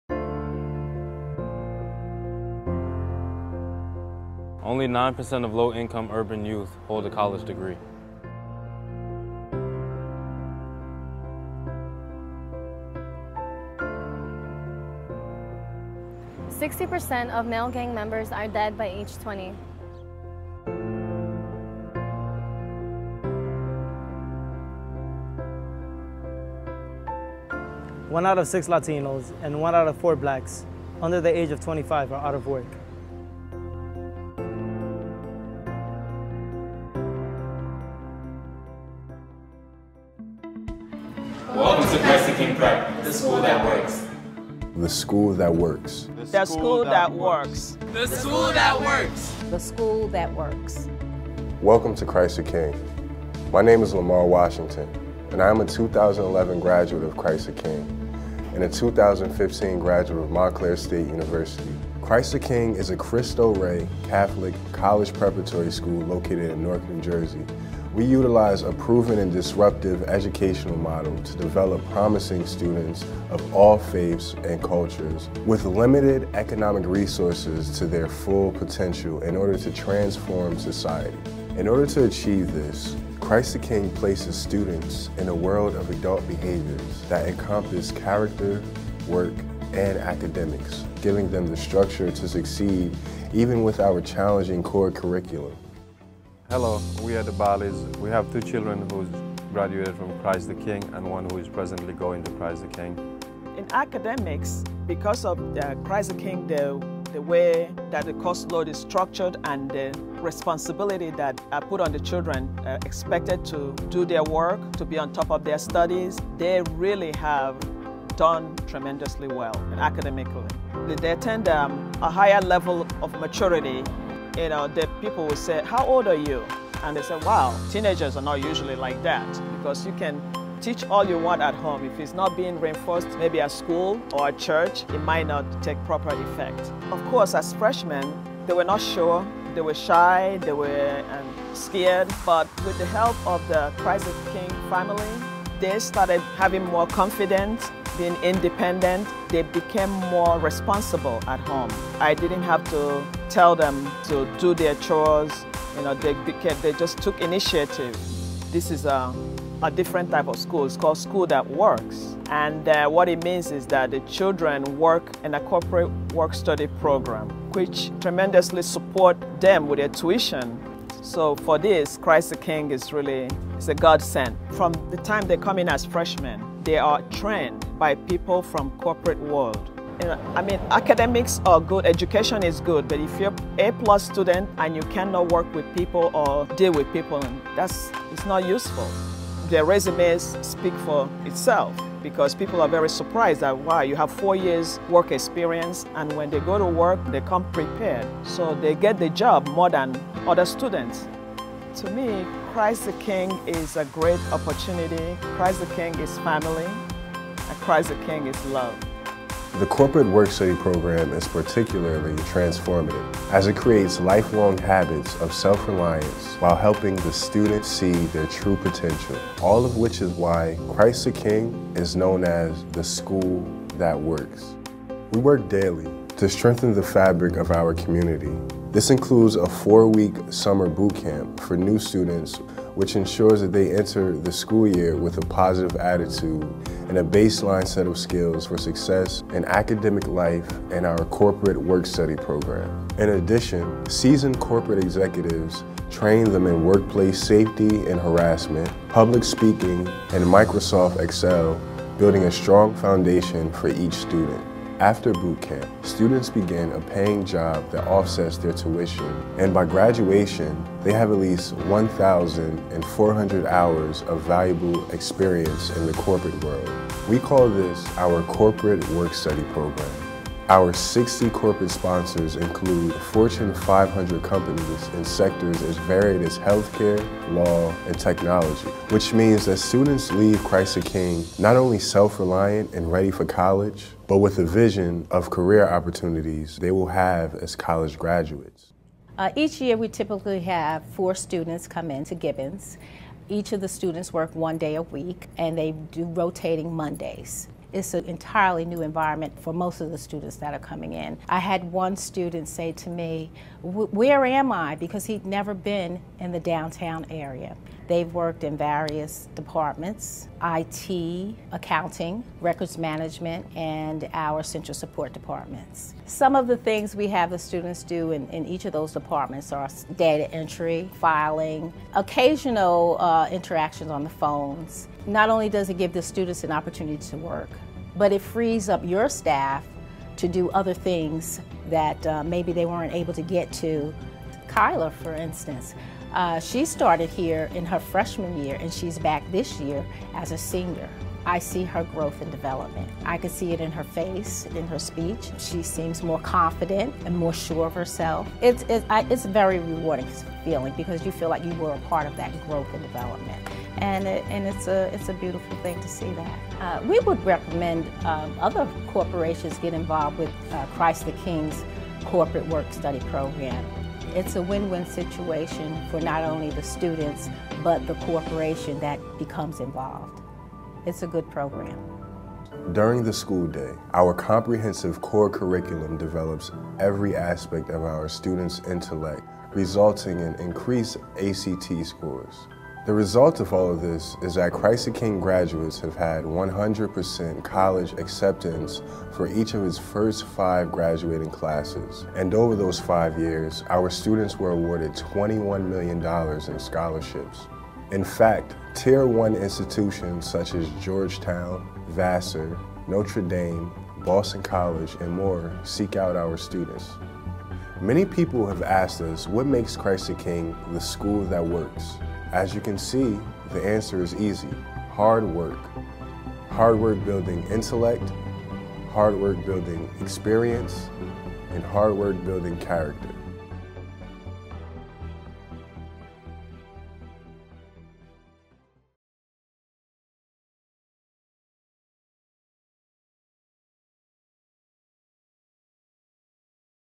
Christ the King Prep - The School That Works!
A member of the Cristo Rey Network of 30 schools nationally and growing, Christ the King Preparatory School is located in Newark, NJ. We are a coeducational, Catholic, college prep high school that provides a quality education for urban youth with limited economic resources and educational options.  Students of all faiths and cultural heritages are welcome.

We promote academic achievement, professionalism and virtuous character through a challenging core curriculum and innovative work-study program.  This Corporate Work Study Program provides a diversity pipeline for companies that wish to satisfy their philanthropic spirit and engage in social responsibility. Corporations that participate in our internship program see value added to their triple bottom line. In turn, students gain 1400 hours of valuable corporate work experience during their four years here. All with the purpose of transforming  society.

Take a look at why we are The School That Works!